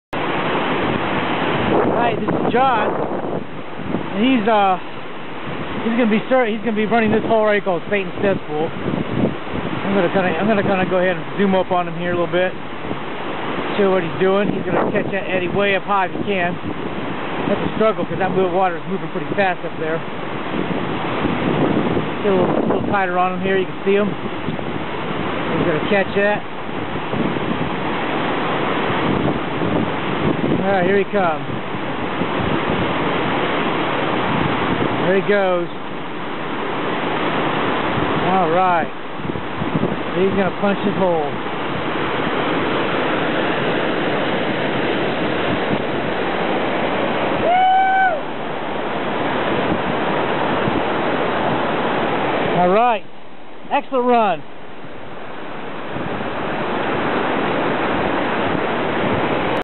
John runs Satans Cesspool for the first time
John had a blast running this rapid.